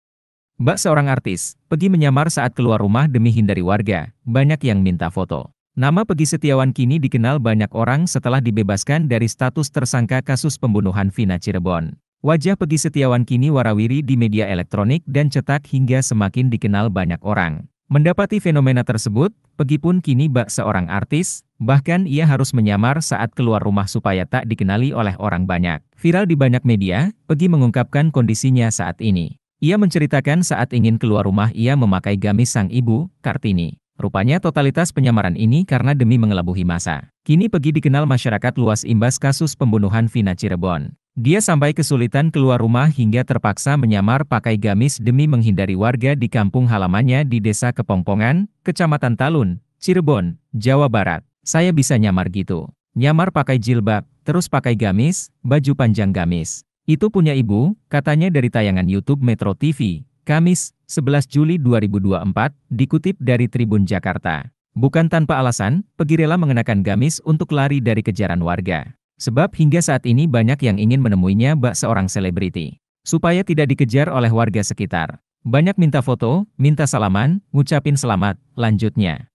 Bak Seorang Artis, Pegi Menyamar Saat Keluar Rumah Demi Hindari Warga: Banyak yang Minta Foto
Nama Pegi Setiawan kini dikenal banyak orang setelah dibebaskan dari status tersangka kasus pembunuhan Vina Cirebon.
Wajah Pegi Setiawan kini wara-wiri di media elektronik dancetak hingga semakin dikenal banyak orang.

Program: -
Host: -
Editor Video: Muhammad Maulana Ahmad Al H
Uploader: -